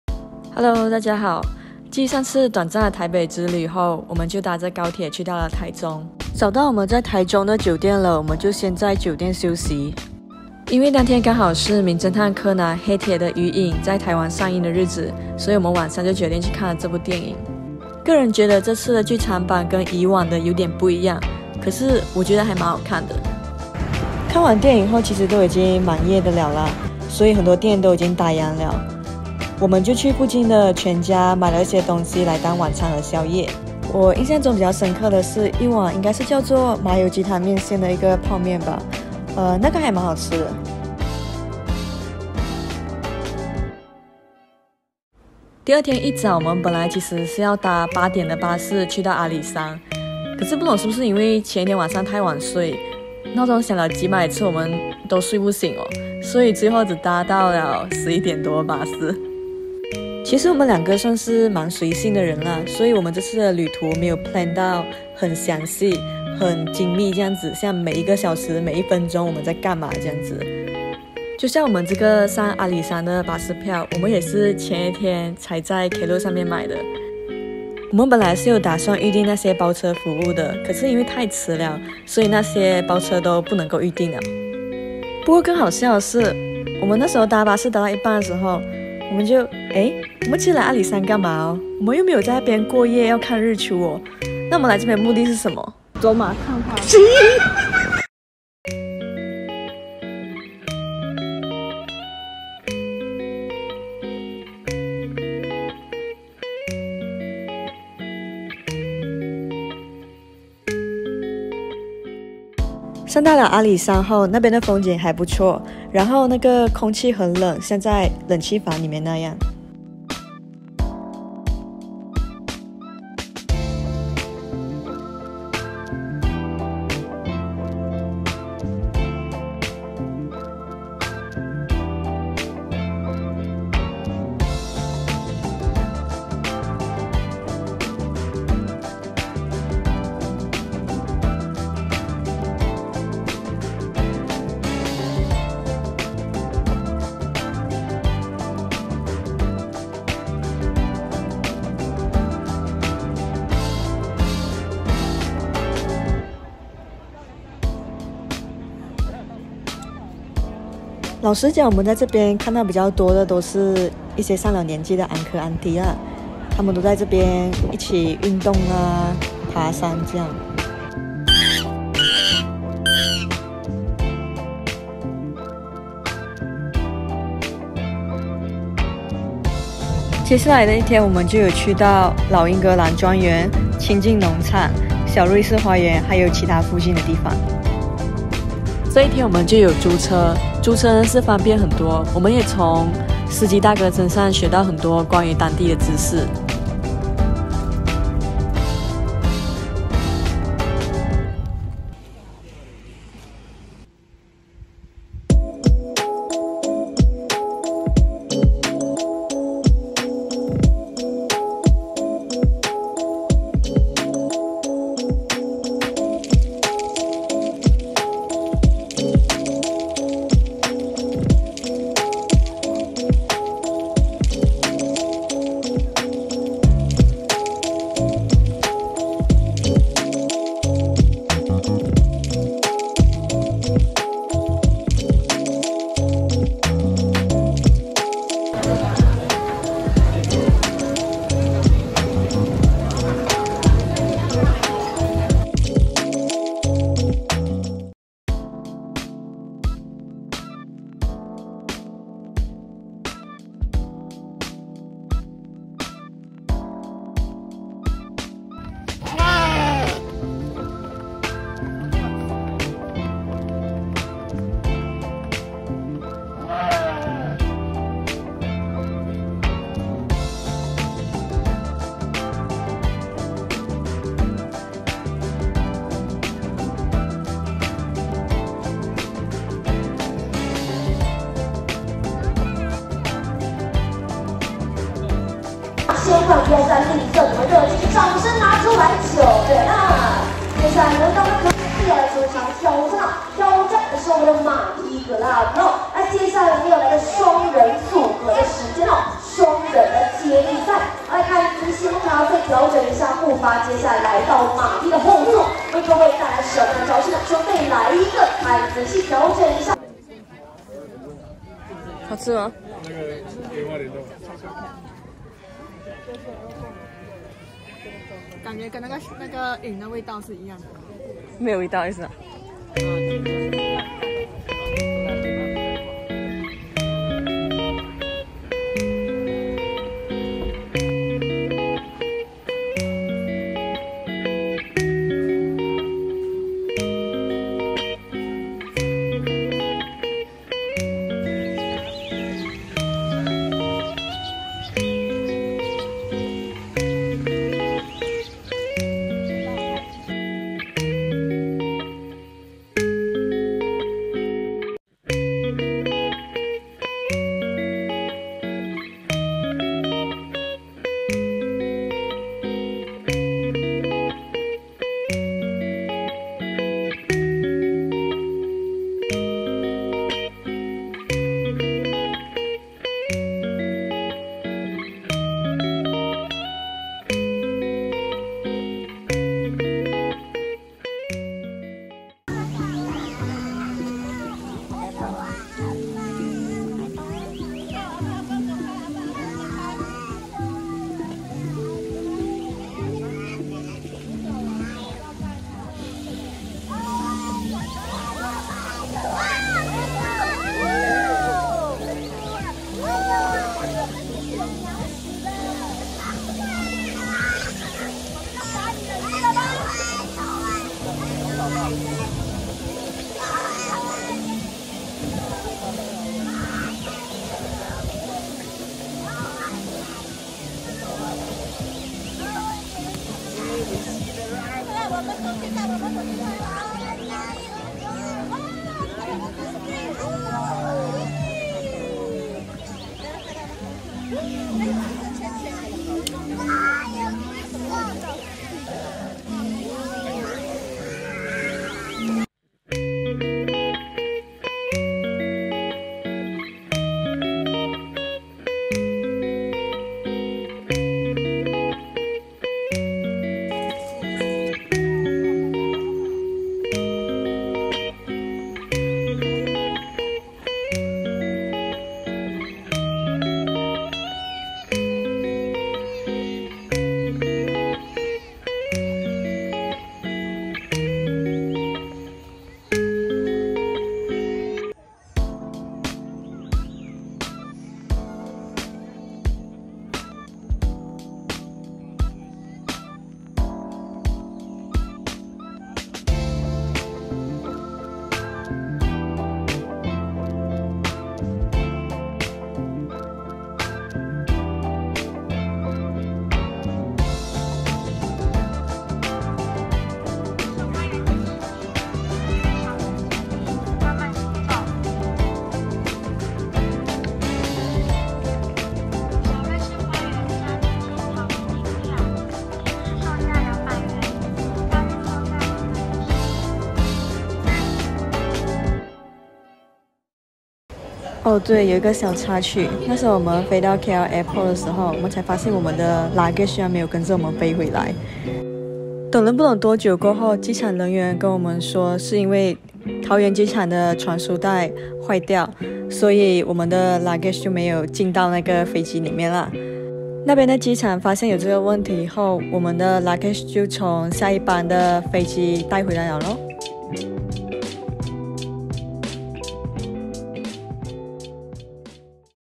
台灣旅遊vlog 2: 抵達臺中 | 清境農場｜老英格蘭莊園｜小瑞士花園｜紙箱王創意園區 | 阿里山
遥控器 / Remote control:
0:00 抵达台中 / Arrived at Taichung
0:45 阿里山 / Brief visit to Alishan
3:00 老英格兰庄园 / The Old England Manor
4:05 清境农场 / Qingjing Farm
5:25 纸箱王创意园区 / Carton King Creativity Park
5:41 小瑞士花园 / Swiss Garden
8:40 回程&找回行李🥲 / Going back & Finding lost luggage :')